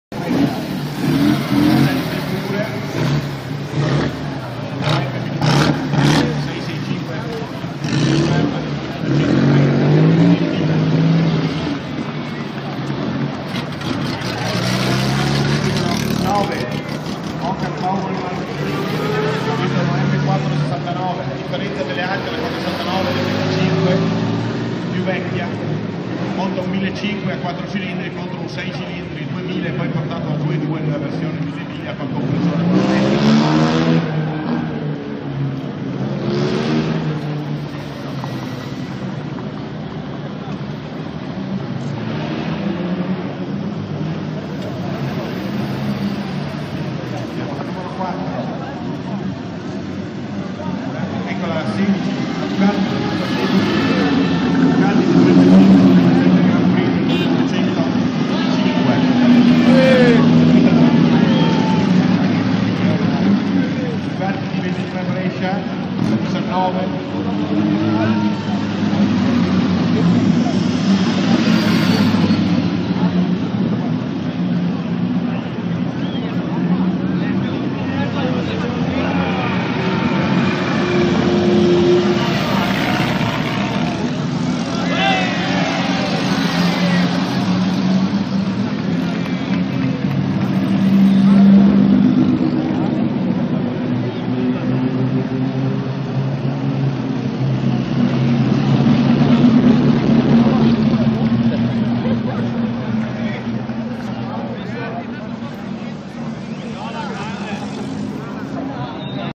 LA 1000 MIGLIA FA TAPPA A PADOVA
Per la prima volta la 1000 miglia fa tappa a Padova. Ecco il passaggio delle vetture nella centrale piazza della Frutta. [continua a leggere su